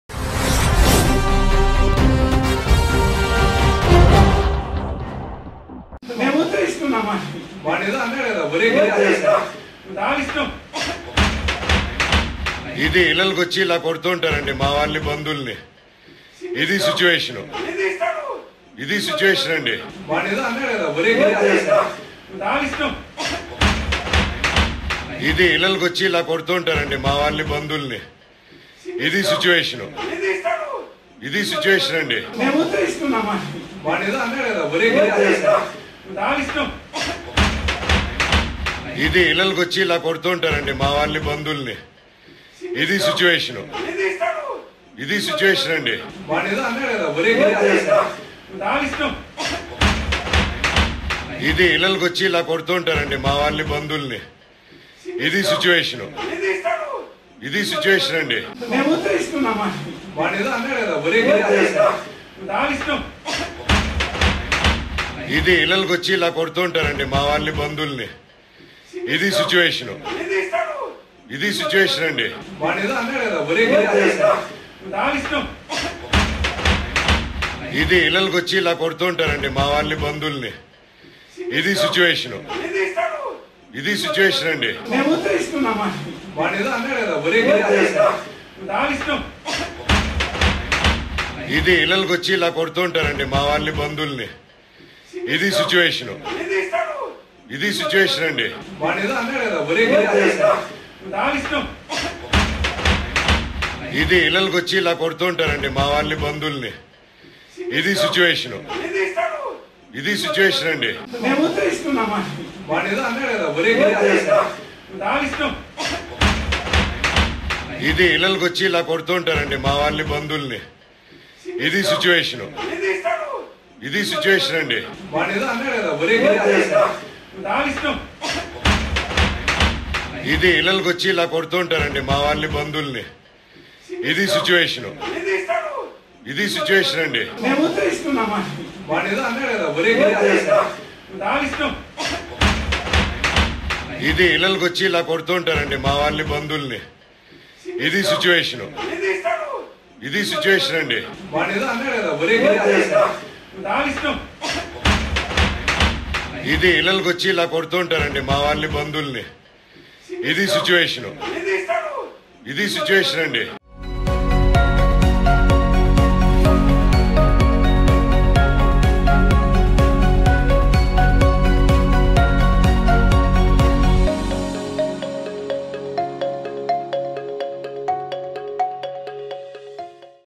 Manchu Vishnu Vs Manoj || రోడ్డుకెక్కిన మంచు ఫ్యామిలీ.. తమ్ముడు ఇంటిపై విష్ణు దాడి
Actor Manchu Mohan Babu's children, half-brothers and actors Manchu Vishnu and Manchu Manoj get into a fight at an unknown place. Manchu Manoj posts a video on Facebook accusing his brother Manchu Vishnu’s misbehaviour with his friends & family members.

అన్నదమ్ముల మధ్య గొడవలు సాధారణమే. కానీ సెలబ్రిటీల ఇళ్లల్లో జరిగితే అవి వైరల్ గా మారతాయి. గత కొన్నాళ్లుగా మంచు మనోజ్ కి, విష్ణుకి మధ్య మాటలు లేవని తెలుస్తుంది. ఇటీవల మనోజ్ పెళ్ళికి కూడా విష్ణు రాకపోవడం, మోహన్ బాబు యూనివర్సిటీ వార్షికోత్సవంలో మనోజ్, విష్ణు మాట్లాడుకోకపోవడంతో వీరిద్దరి మధ్య ఏదో గొడవ అవుతుందని వార్తలు వచ్చాయి